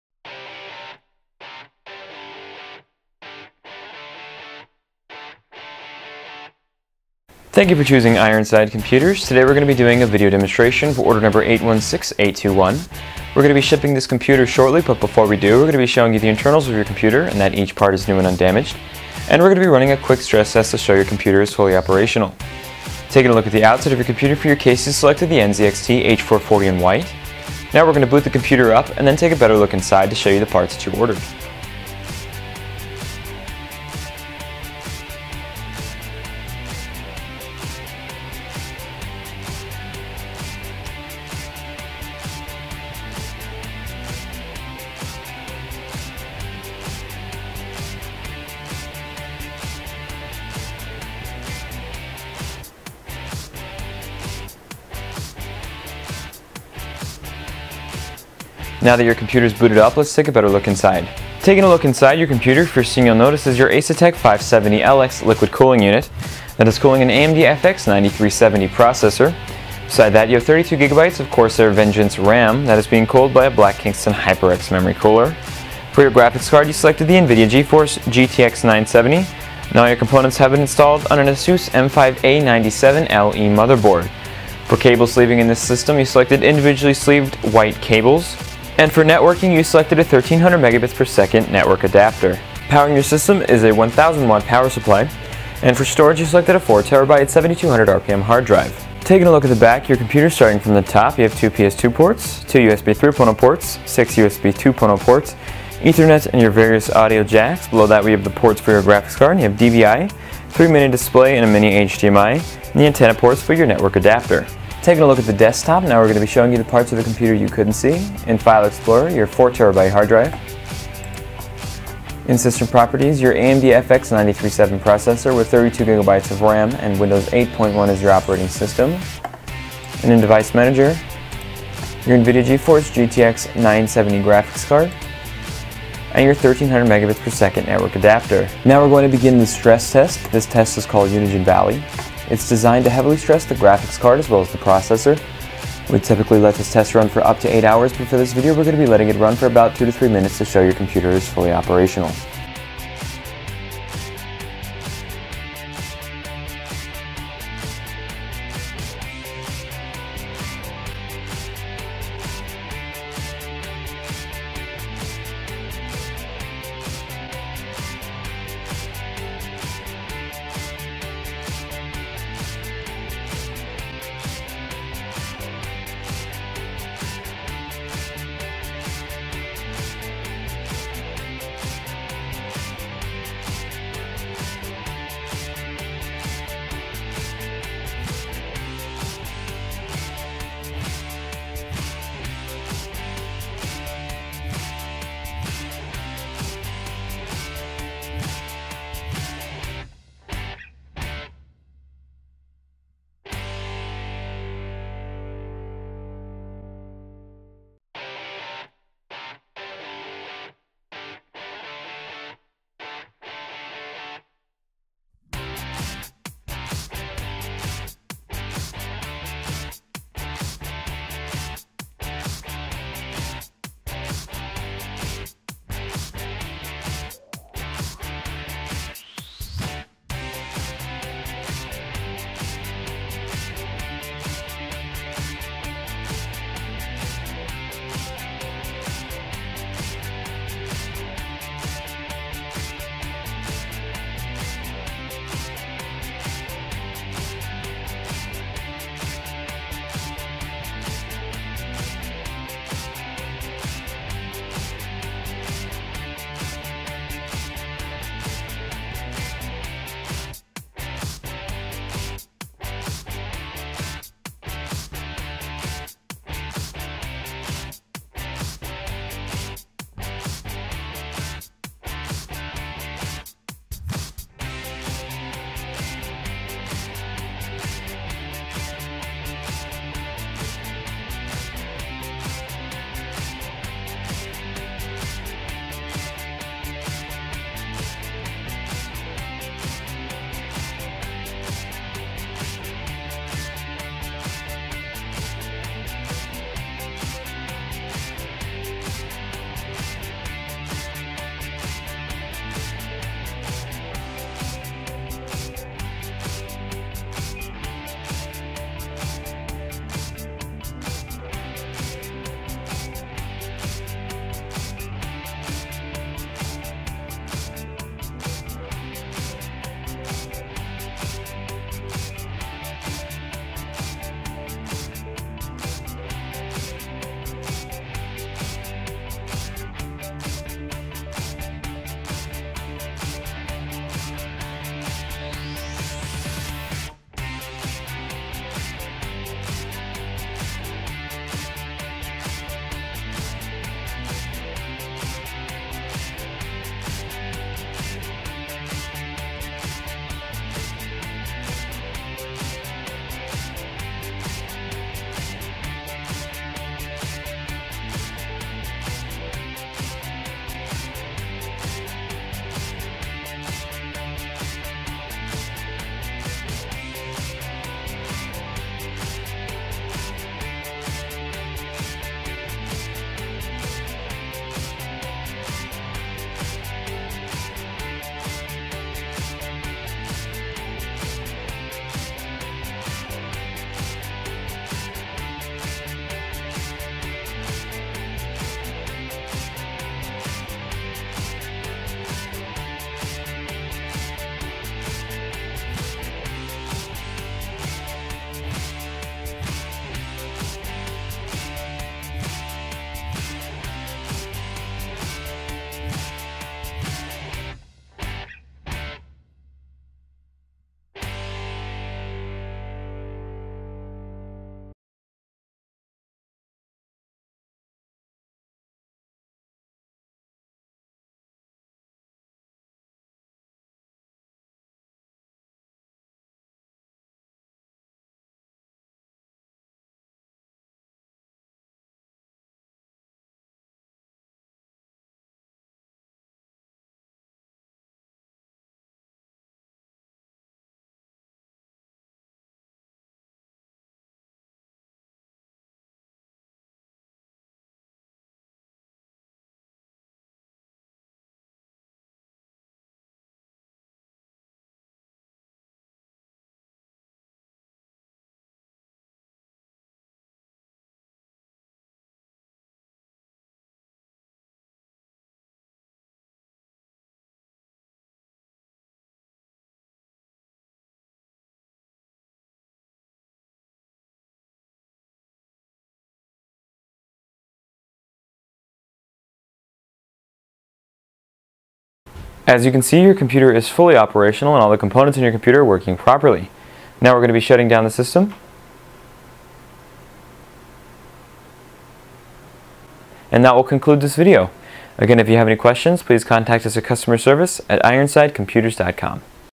Ironside Computers Video Demonstration - Order 816821
PC Specifications:
Case:  H440 White
Processor: AMD FX-9370
CPU Cooling:  Asetek 570LX High Performance Liquid Cooling
Motherboard: ASUS M5A97 LE
Memory: 32GB 1600MHz
Graphics Card: Nvidia Geforce GTX 970
Hard Drive:  4TB 7200 RPM
Power Supply: Standard 1000 Watt
OS: Windows 8.1